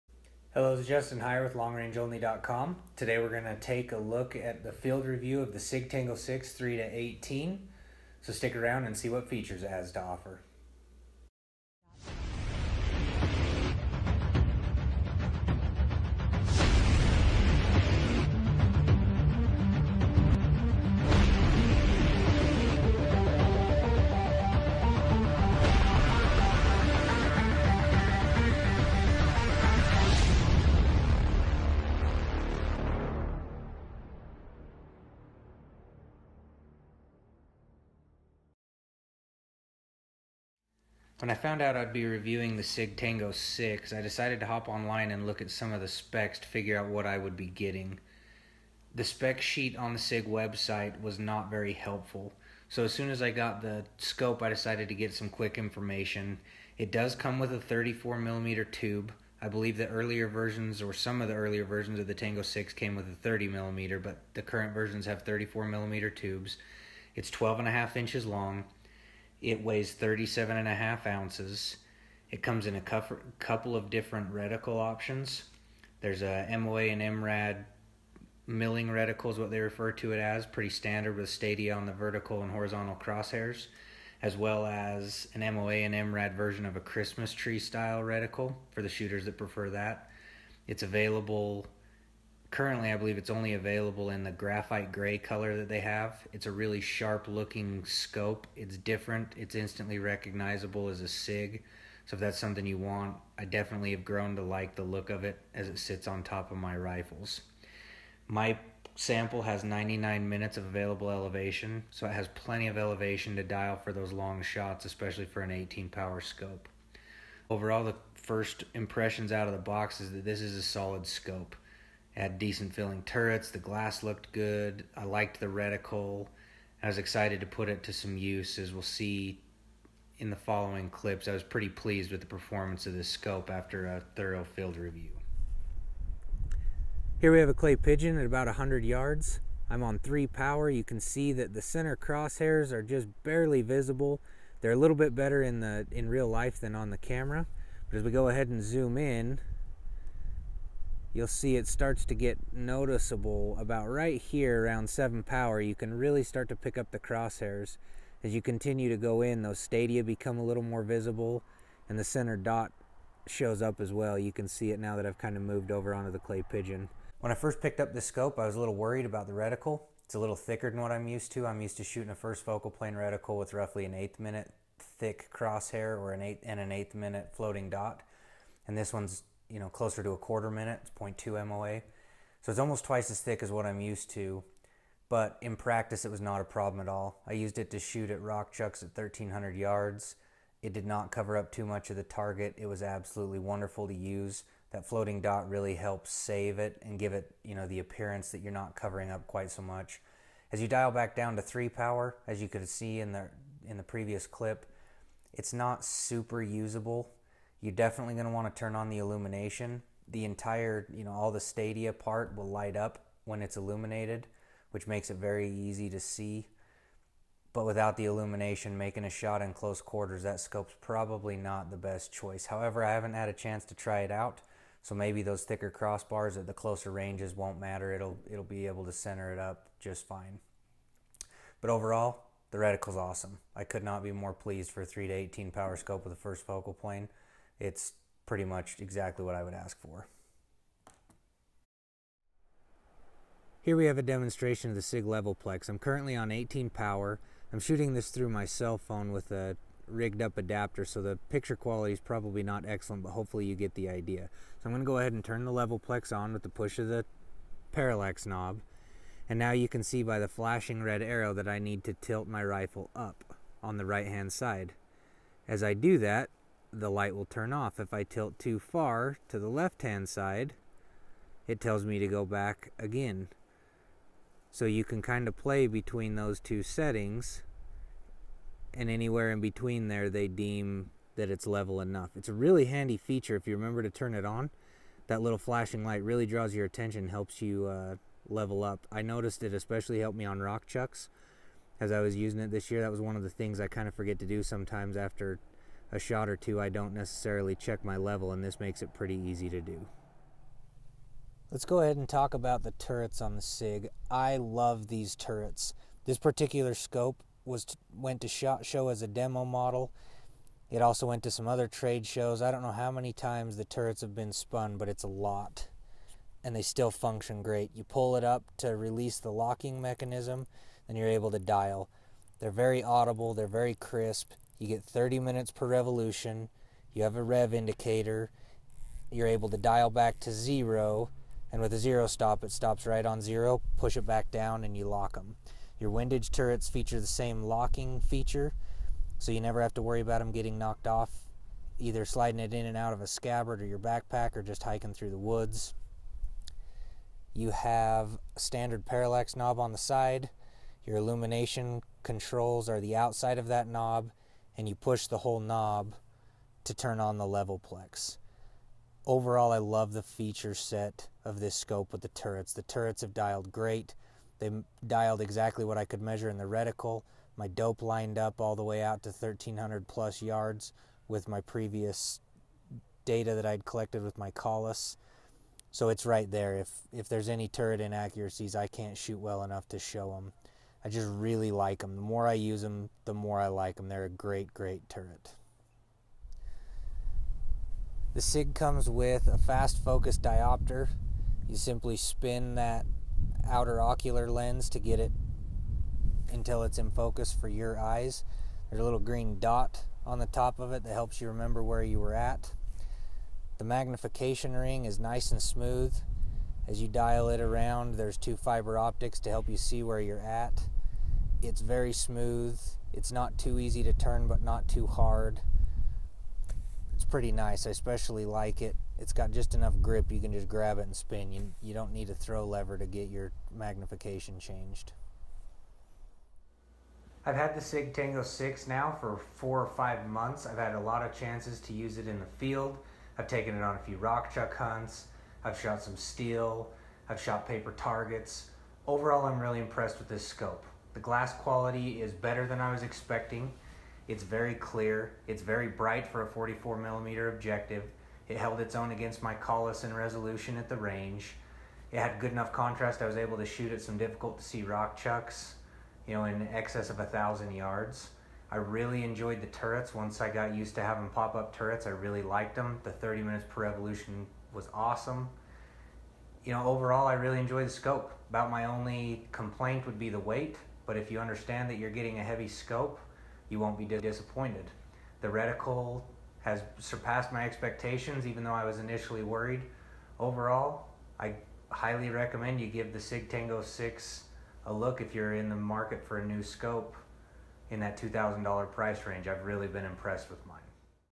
Sig Sauer Tango 6 3-18x44 Review
Here LRO editor Justin Hyer does a full video review of the Sig Sauer Tango 6 FFP rifle scope in 3-18x44